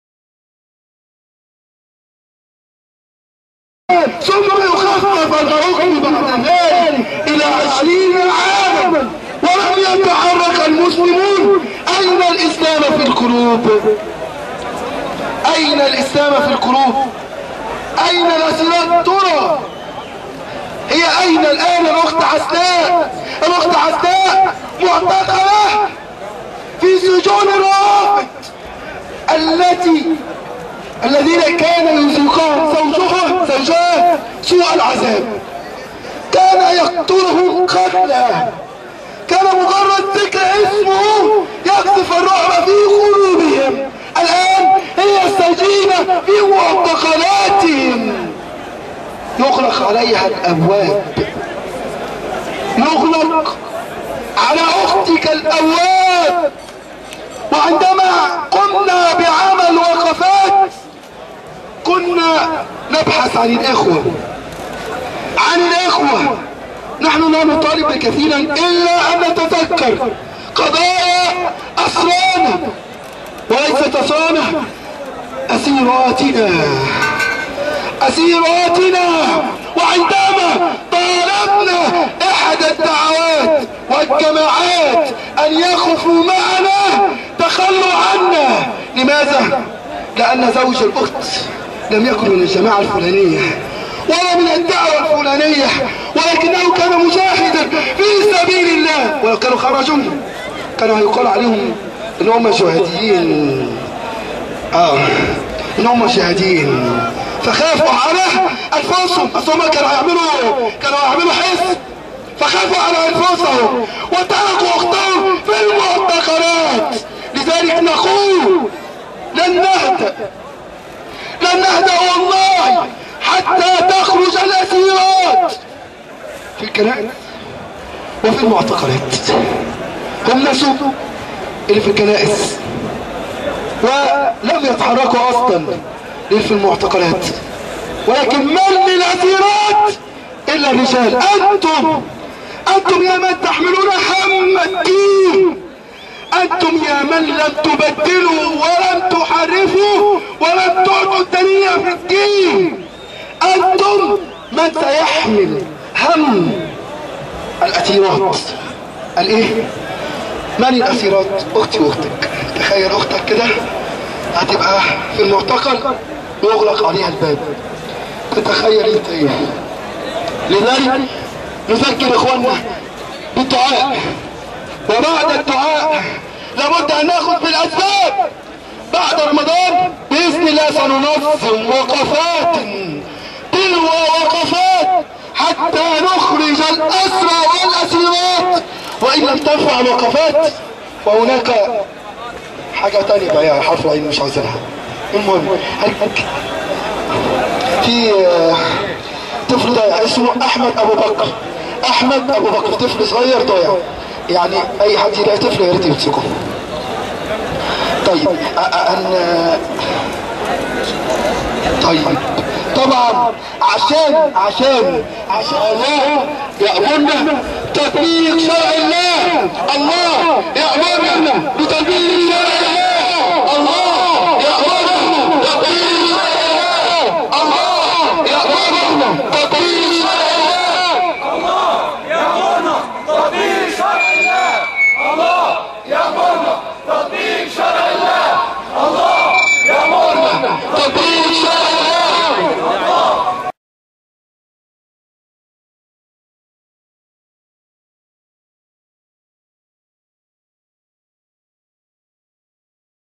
الإسلاميون يطالبوا من التحرير بالإفراج عن السياسيين
الإسلاميون يطالبوا بالإفراج عن السياسيين فى جمعة وحدة الصف فى التحرير